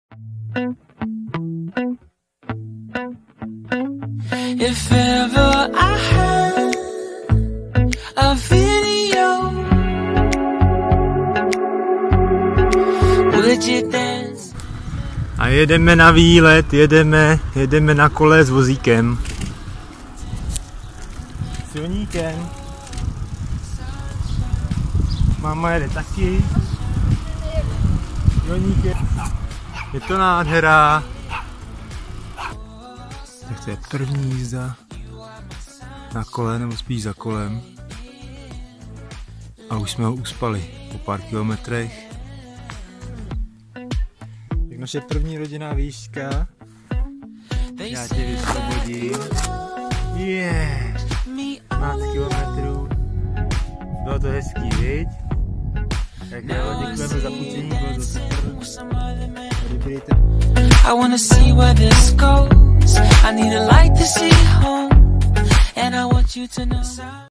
První rodinná vyjížďka na kole
Zkouška jestli dítě vydrží v cruiseru dopadla dobře za chvilku usnul :-)
Cesta vedla po silnici mezi vesnicemi kde je minimální provoz jinam bych s ním zatím nejel.